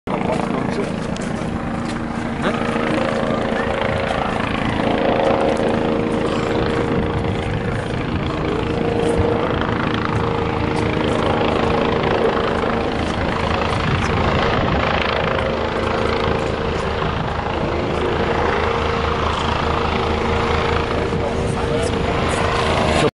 Löschwasserabwurf Polizeihubschrauber Bambi-Packet 4-2014
Hubschrauber der hess. Polizei wirft im Rahmen einer Waldbrandübung im TrÜbPl Wildflecken sein Löschwasser des Außenlastbehälters auf den angenommenen Brandherd.